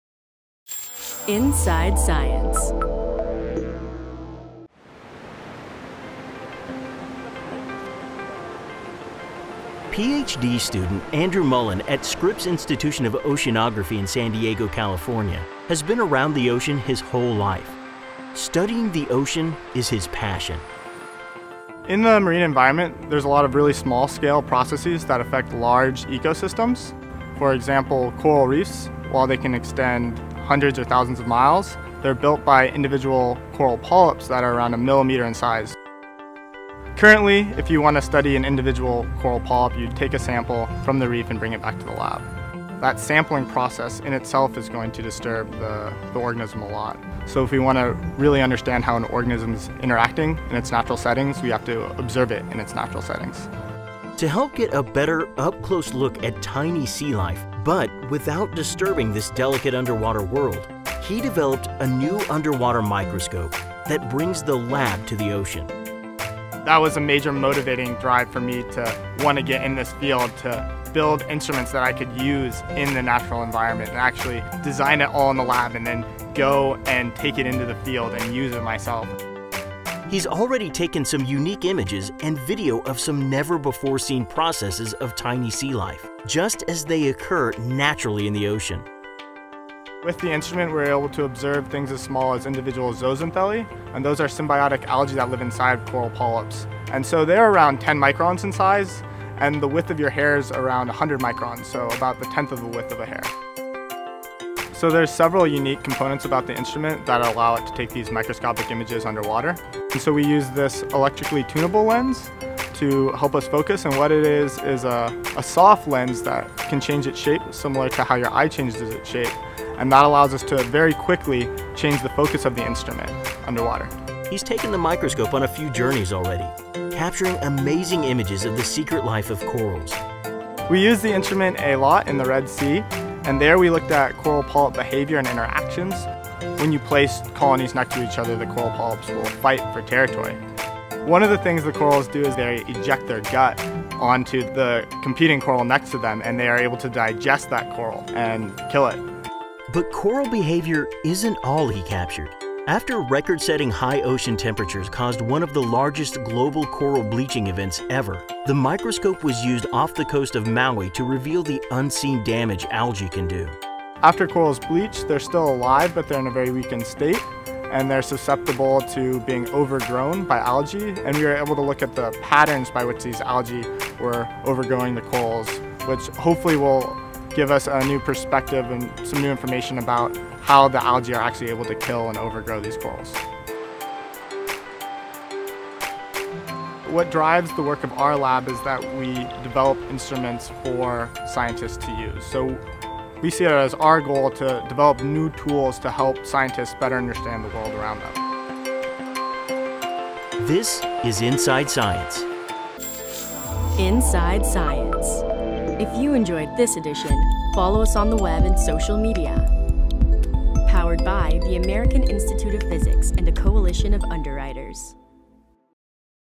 Unique Underwater Microscope
Andrew Mullen, Ph.D student at Scripps Institution of Oceanography in San Diego, California, has been around the ocean his whole life. He grew up along the California coast, surfing waves, soaking up the sun and living the dream. Studying the ocean is his passion.

“In the marine environment, there’s a lot of really small scale processes that affect large ecosystems. For example, coral reefs, while they can extend hundreds or thousands of miles, they’re built by individual coral polyps that are around a millimeter in size,” said Mullen.

According to Mullen, “Currently, if you want to study an individual coral polyp, you take a sample from the reef and bring it back to the lab. But, that sampling process in itself is going to disturb the organism a lot. So if we want to really understand how an organism’s interacting in its natural settings, we have to observe it in its natural settings.”

To help get a better, up-close look at tiny sea life -- but without disturbing this delicate underwater world -- he developed a new underwater microscope that brings the lab to the ocean.

“That was a major motivating drive for me to want to get in this field, to build instruments that I could use in the natural environment. And actually get to design it all in the lab and then go and take it into the field and use it myself,” said Mullen.

He's already taken some unique images and video of some never-before-seen processes of tiny sea life, just as they occur natural in the ocean.

“With the instrument we’re able to observe things as small as individual zooxanthellae, and those are symbiotic algae that live inside coral polyps. And so they are around ten microns in size, and the width of your hair is around a hundred microns, so about the tenth of the width of a hair,” stated Mullen.

He also stated, “There are several unique components about the instrument that allow it to take these microscopic images underwater. We use this electrically tunable lens to help us focus. And what that is, is a soft lens that can change its shape, similar to how your eye changes its shape, and that allows us to very quickly change the focus of the instrument underwater.”

He's taken the microscope on a few journeys already, capturing amazing images of the secret life of corals.

“We use the instrument a lot in the Red Sea and there we looked at coral polyp behavior and interactions. When you place colonies next to each other the coral polyps will fight for territory. One of the things the corals do, they eject their gut onto the competing coral next to them and they are able to digest that coral and kill it,” said Mullen.

But coral behavior isn't all he captured. After record-setting high ocean temperatures caused one of the largest global coral-bleaching events ever, the microscope was used off the coast of Maui to reveal the unseen damage algae can do.

“After corals bleach they’re still alive, but they are in a very weakened state, and they are susceptible to being overgrown by algae. And we were able to look at patterns by these algae were overgrowing the corals. Which hopefully will give us a new perspective and some new information about how the algae are actually able to kill and overgrow these corals.”

Finally Mullen said, “What drives the work of our lab, is that we develop instruments for scientists to use. So, we see it as our goal to develop new tools to help scientists better understand the world around them.”